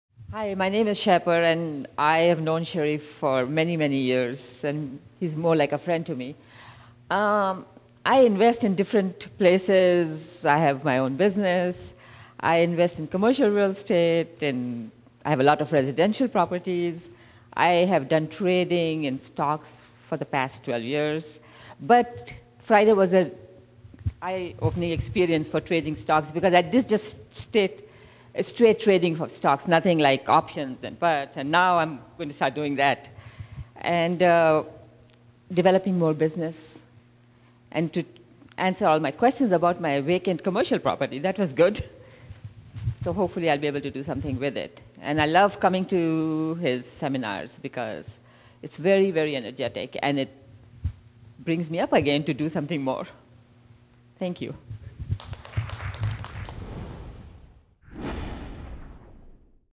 I Love Coming to Cherif's Seminars
Shahper K. shares her appreciation of Cherif Medawar at a recent Cherif Medawar event that teaches about multiple streams of income including Securities Trading, Business, and Commercial Real Estate.